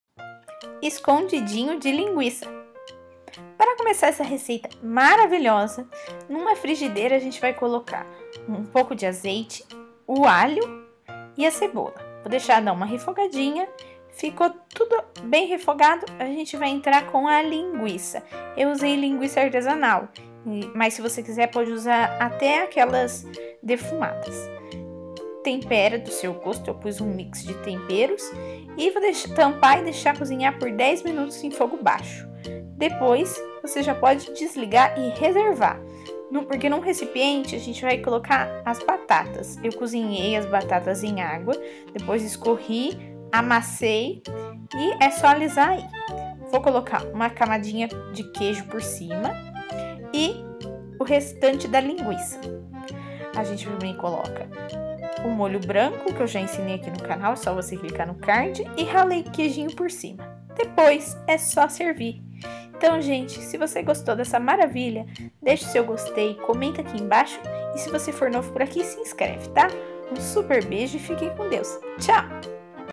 COMO FAZER ESCONDIDINHO DE LIGUIÇA E BATATA
A receita de hoje é um escondidinho de linguiça e batata que leva molho branco! Fica incrível! Venha aprender comigo? Gostou?

Para quem precisar dos Ingredientes:
4 batatas cozidas e amassadas
3 linguiças sem pele
Mix de temperos
Sal a gosto
1 colher de sopa de azeite
1 colher de sopa de alho
1/2 cebola picada em cubos
Queijo Muçarela a gosto